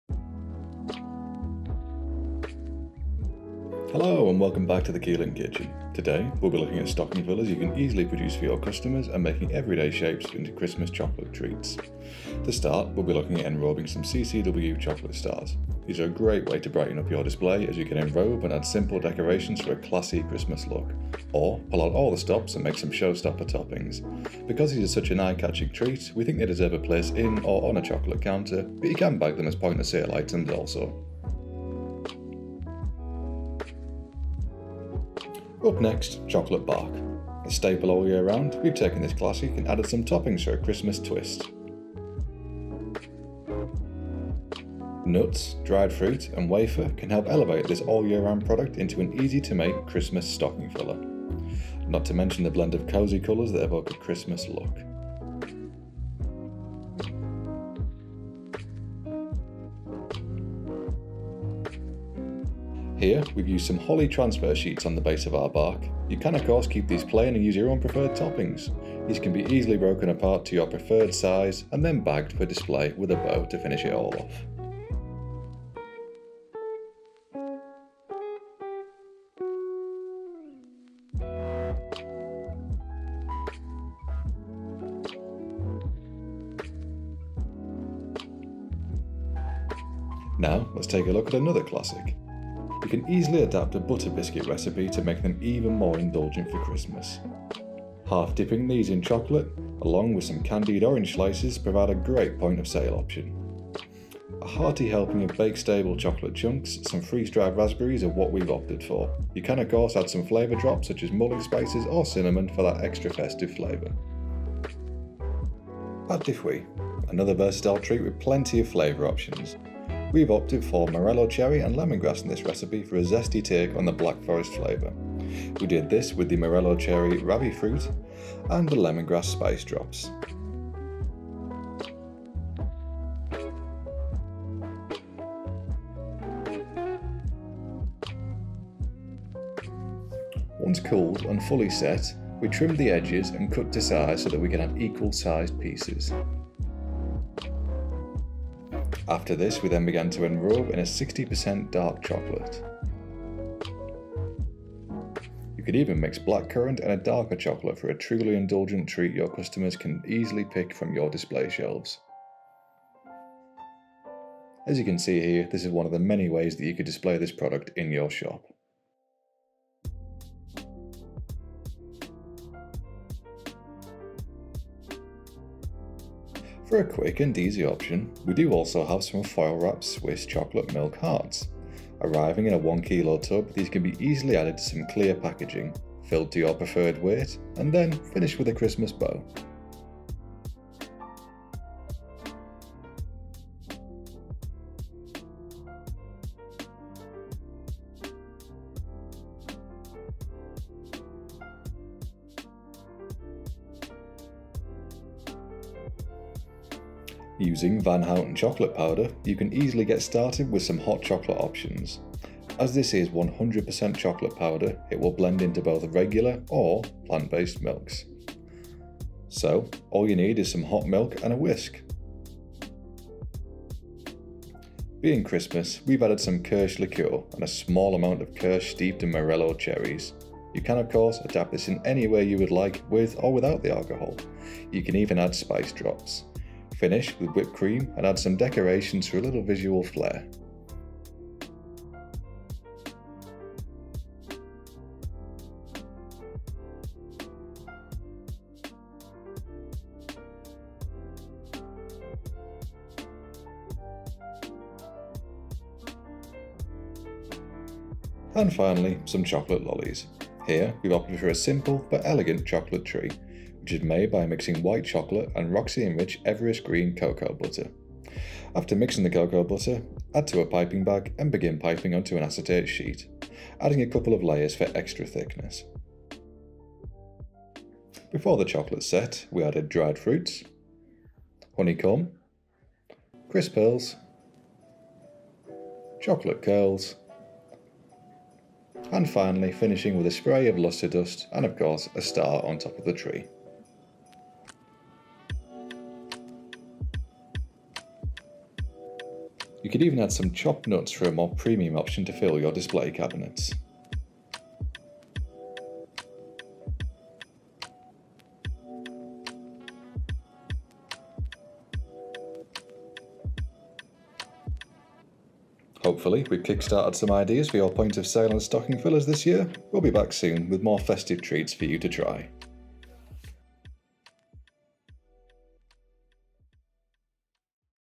Making chocolate stocking fillers and point of sale inspiration | Christmas with Keylink
Cosy up with a cup of hot chocolate and let us inspire your Christmas range with festive point-of-sale products, perfect for stocking fillers!